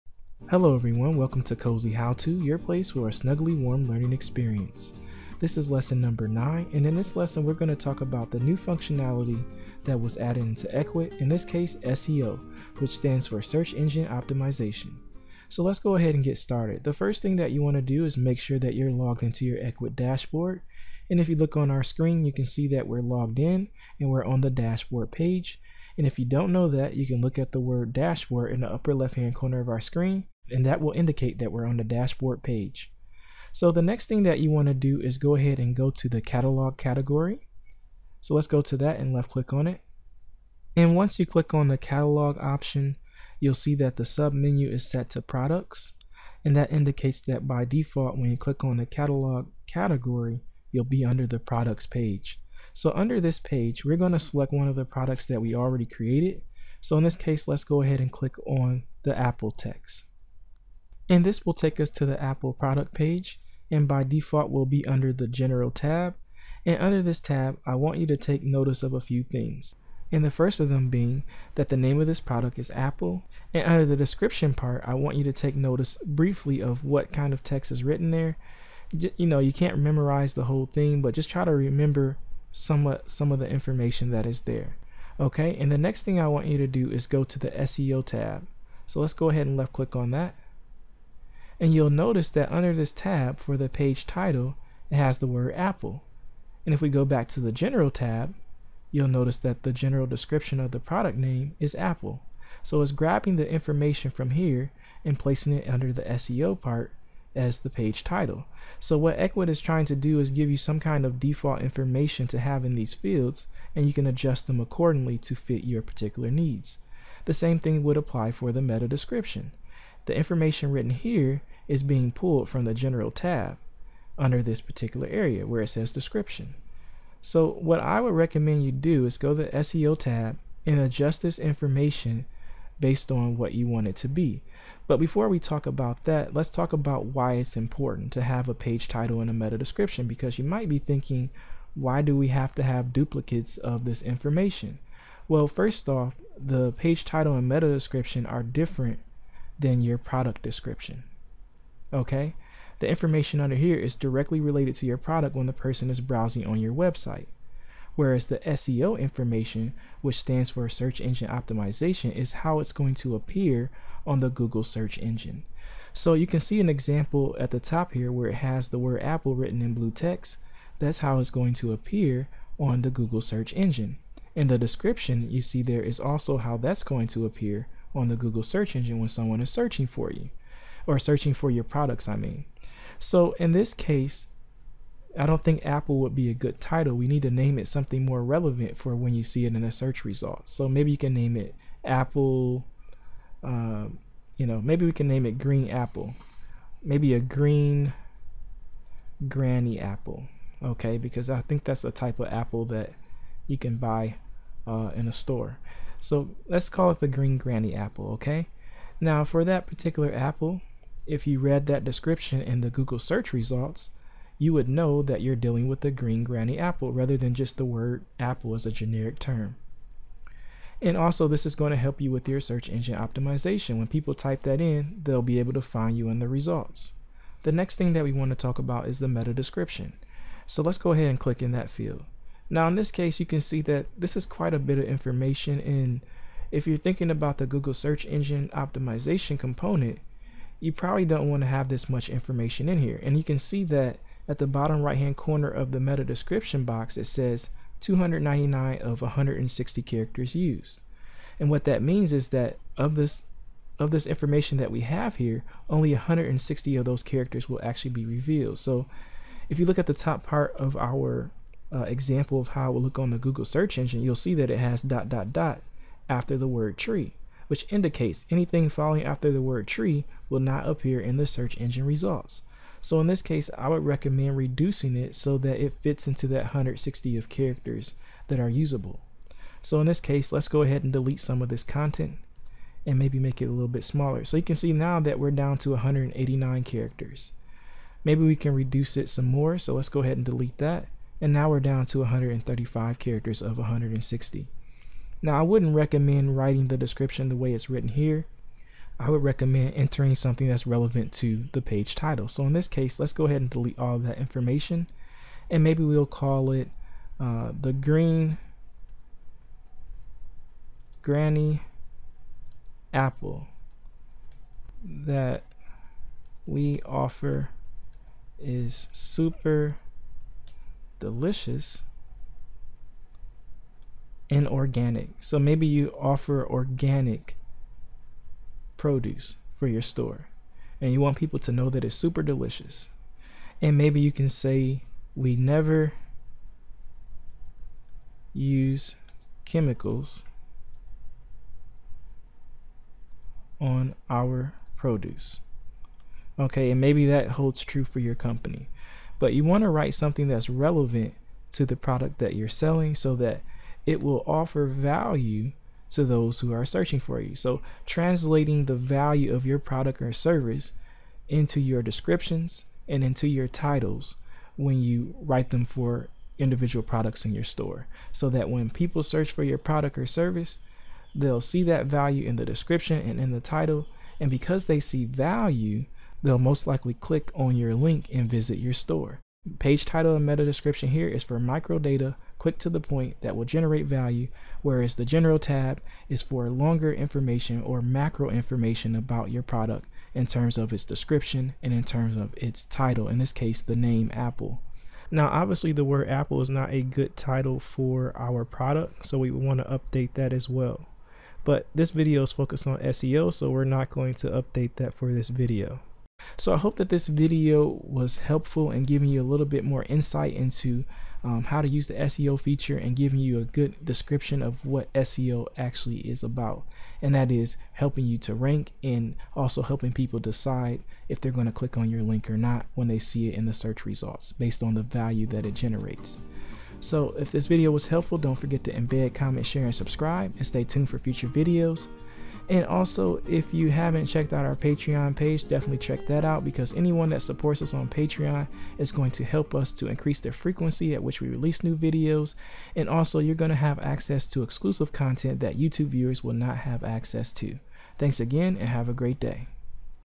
Ecwid: SEO Page Title and Description Lesson 09: by Cozy How To
This is lesson 9 for the Ecwid Shopping Cart software (e-commerce software) and this tutorial will walk you through the process for adding SEO Title and Description. SEO stands for Search Engine Optimization and helps with ranking your content on search engines such as Google.

Also if you have question leave a comment for that as well.